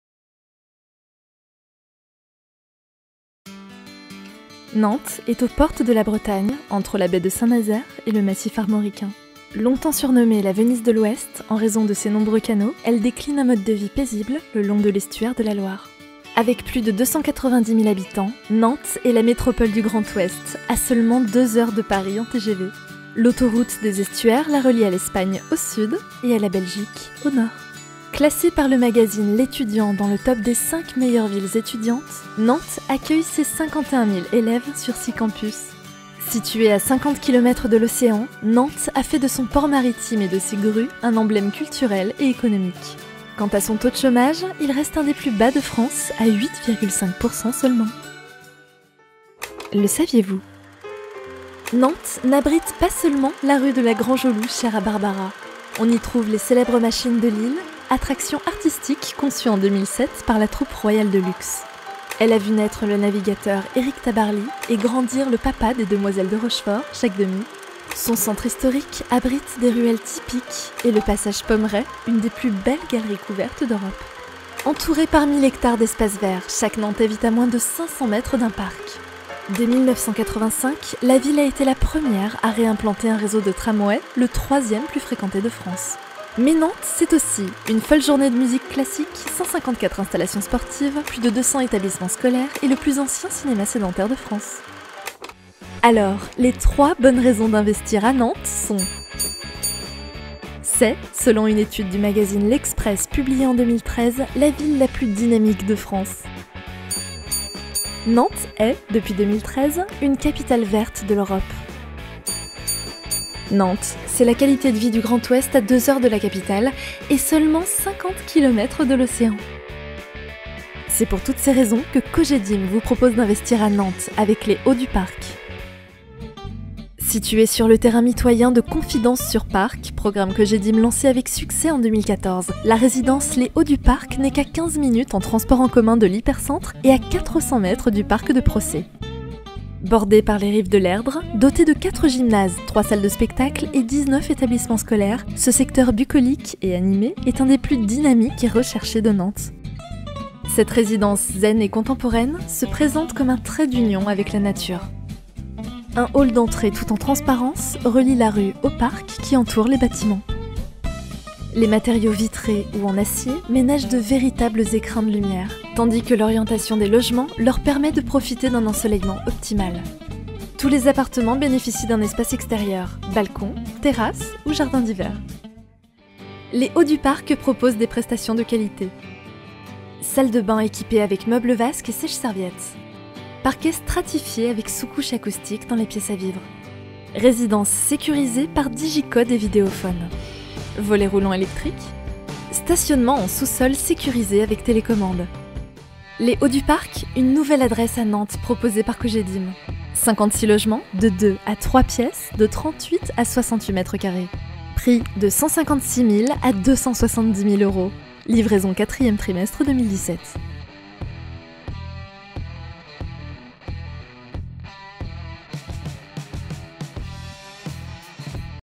Investir à Nantes (44) - Les Hauts du Parc
Découvrez pourquoi investir avec Cogedim à Nantes (44) dans notre résidence Les Hauts du Parc